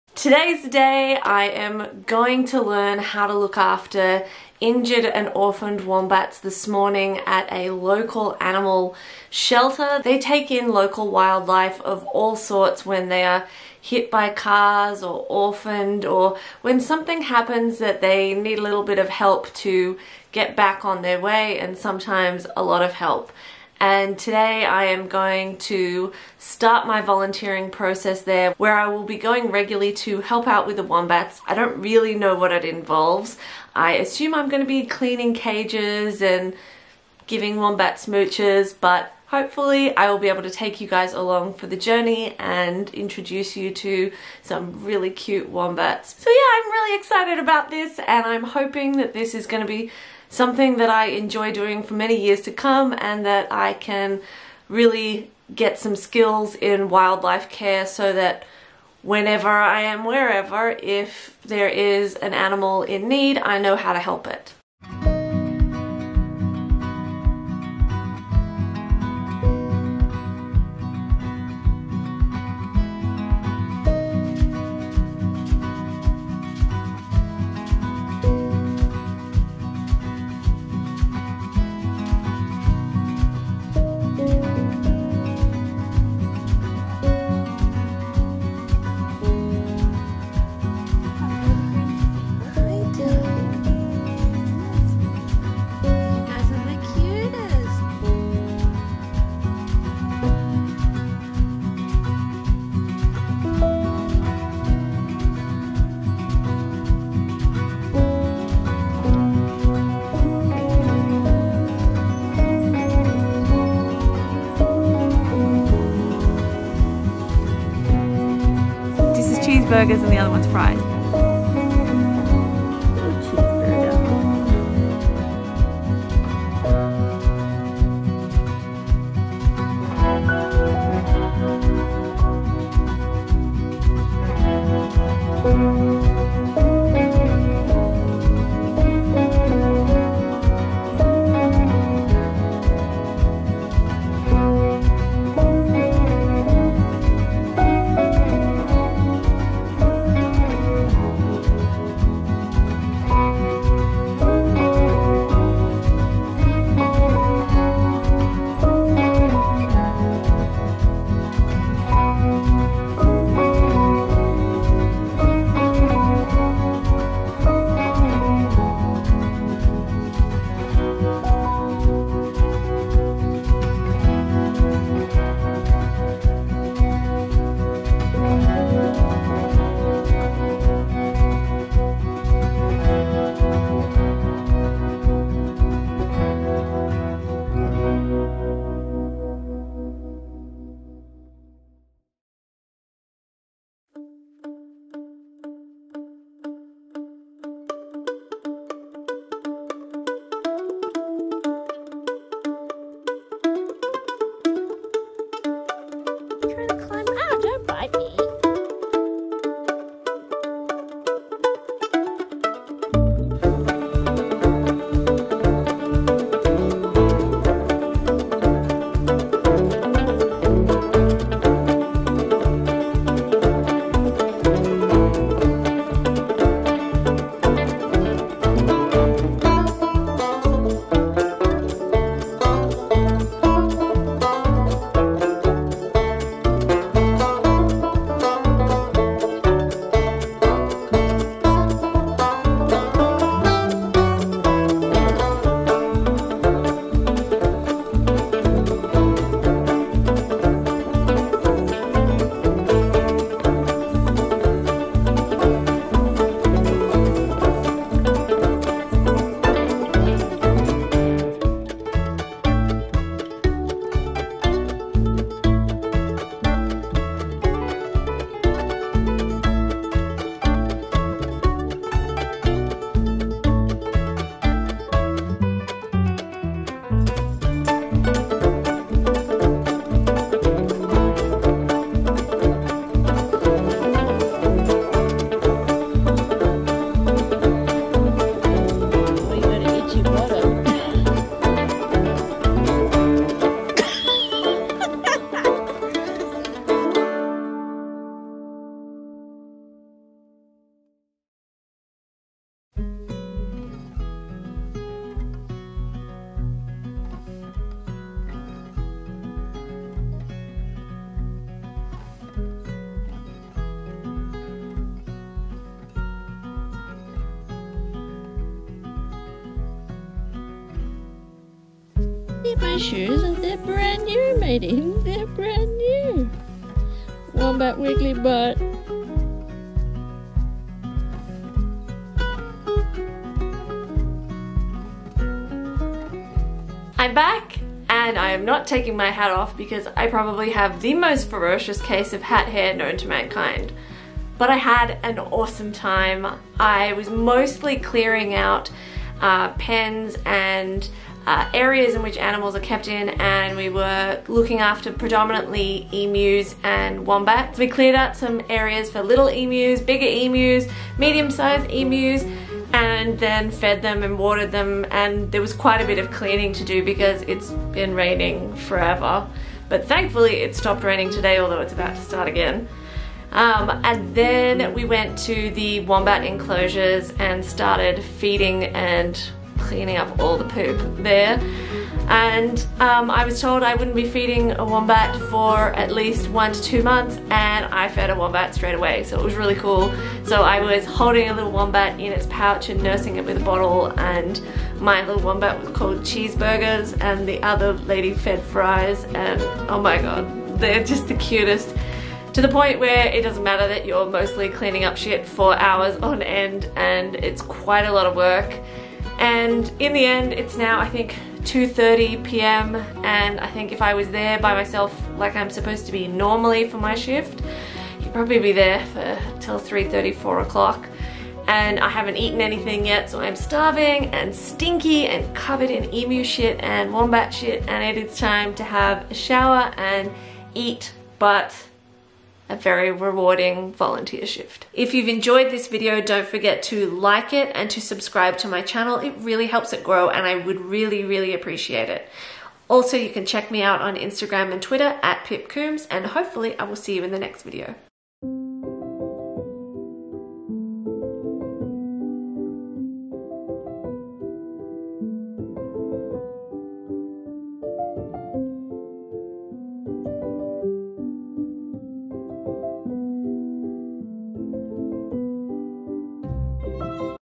Working with wombats - A day in the life vlog
Please make sure you are properly trained before working with wildlife. I have been participating in training with the refuge and Wildlife Rescue South Coast